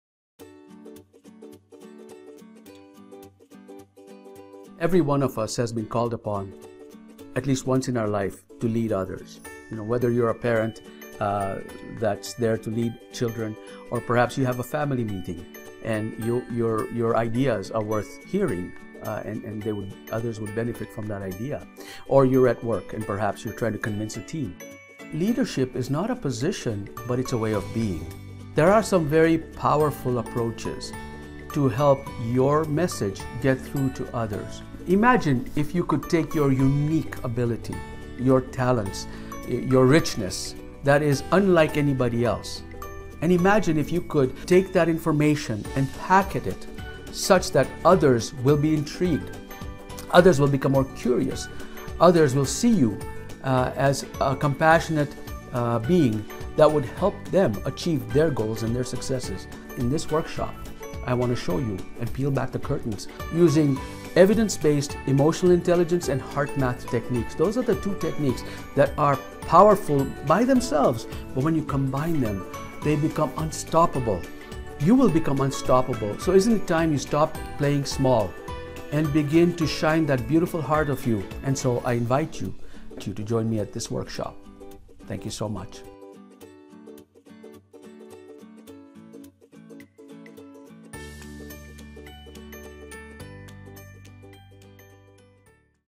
What to Expect: Ultimate Guide to Heart-Centered Leadership
Join Shamir Ladhani and other Kryon team members at the Kryon Mayan Convergence event 2019 December 10 - 17, 2019.  Find out more about Shamir's master class